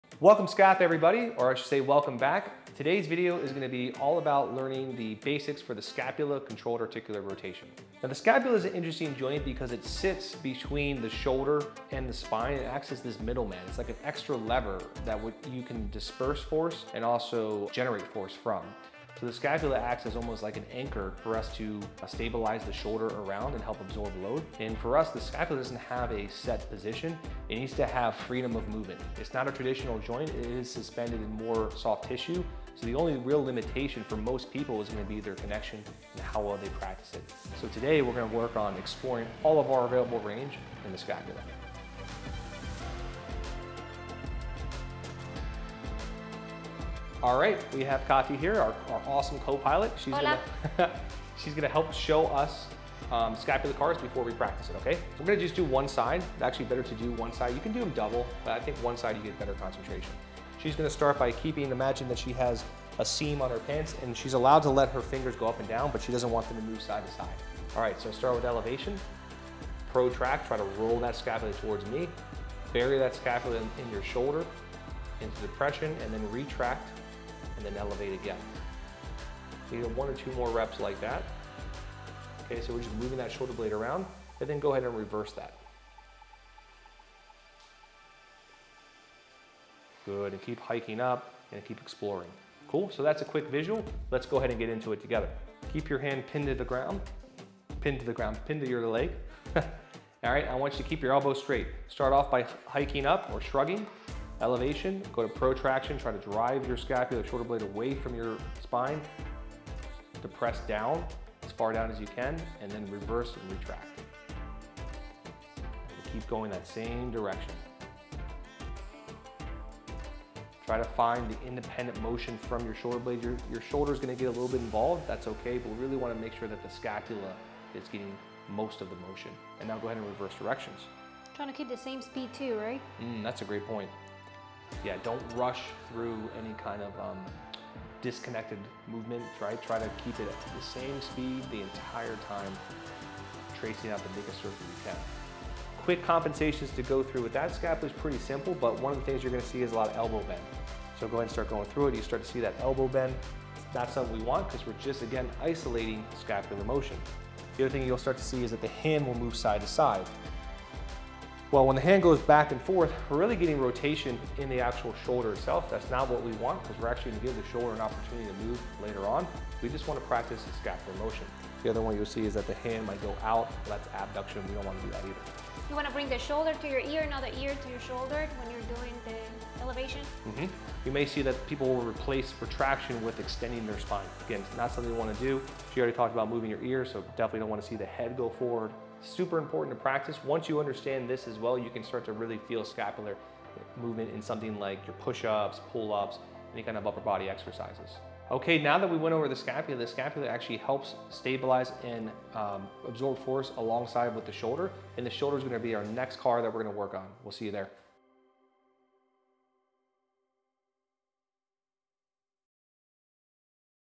How To Do Scapula CARs (Controlled Articular Rotations)
Scapula CARs involve exploring the full spherical range of motion involved in the scapula. By increasing our scapular mobility we improve our potential for absorbing force through the upper limb to the spine. Another benefit people find with Scapular CARs is that they help unlock the thoracic spine by improving the mobility through the shoulder.

Daily CARs practice should be an integral part of rehab, training and injury prevention.
We recommend doing at least 3-5 repetitions in each direction. If the joint feels exceptionally stiff and disconnected, do more repetitions throughout the day. The idea is the more intention and consistency you put into your practice, the more benefits you will get.

Remember CARs are intended to improve the individual building blocks for human movement (joints). By increasing the capacity of a singular joint you exponentially increase the potential for movements involving that joint.

Let us know what future videos you'd like to see!